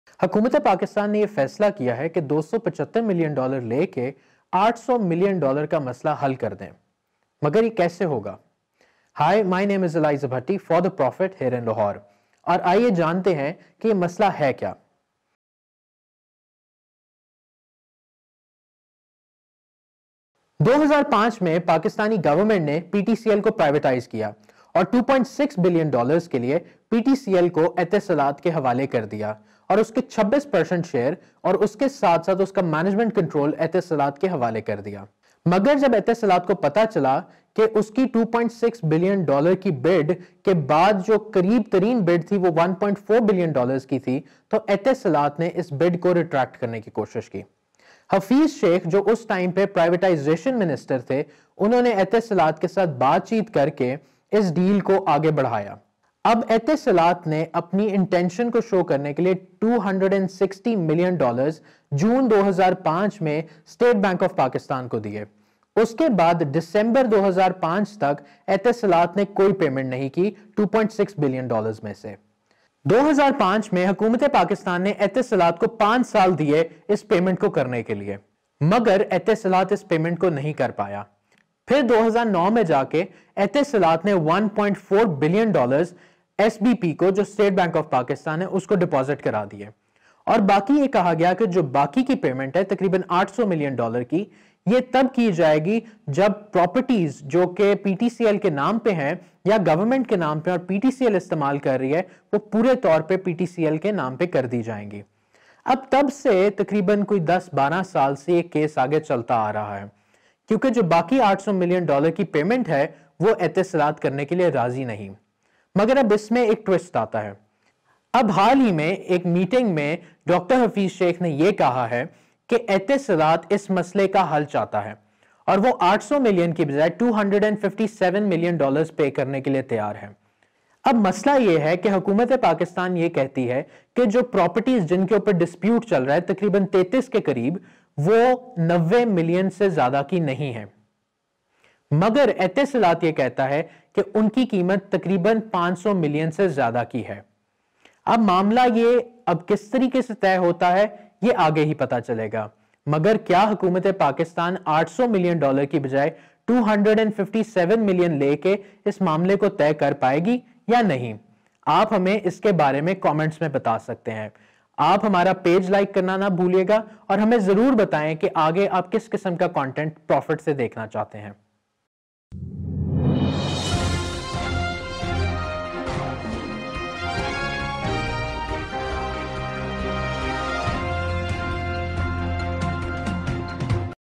Profit Explains: Etisalat - PTCL privatisation dispute
UAE telecom giant Etisalat has submitted to the government a framework of settlement for the long-outstanding payment of $800 million against shares of the Pakistan Telecommunication Company Limited (PTCL).